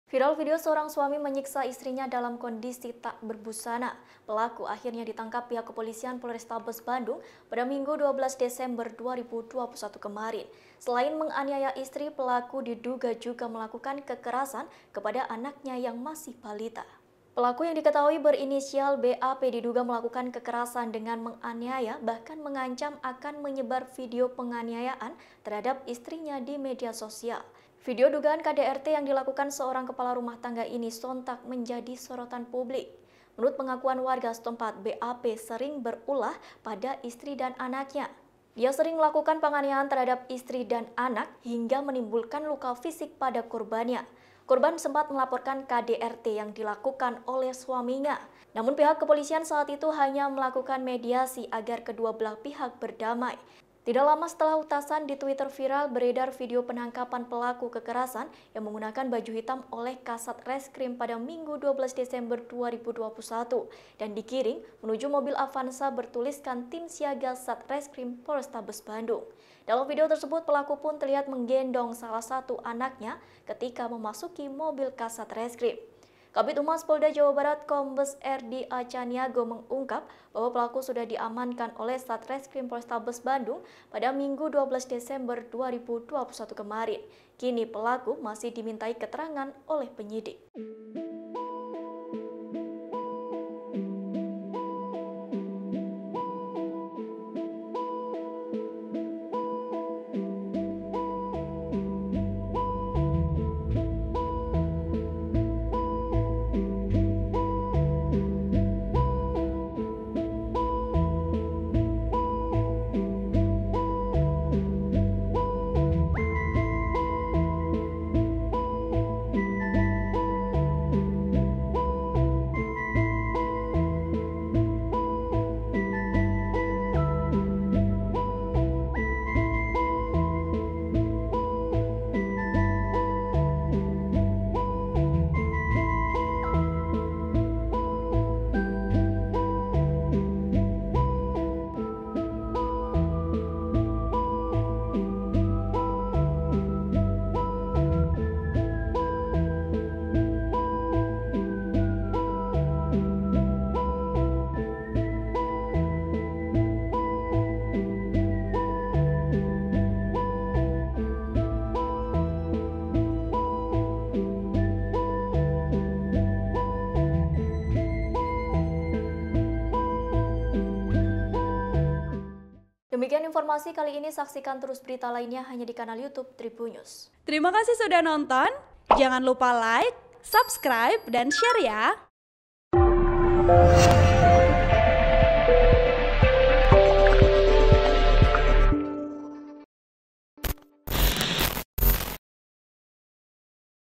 Detik-detik Suami di Bandung Siksa Istri Tanpa Busana, Anaknya Diduga Juga Jadi Korban KDRT
TRIBUN-VIDEO.COM - Viral video seorang suami menyiksa istrinya dalam kondisi tak berbusana.

Pelaku akhirnya ditangkap pihak kepolisian Polrestabes Bandung pada Minggu (12/12/2021).

Selain menganiaya istri, pelaku diduga juga melakukan kekerasan kepada anaknya yang masih balita.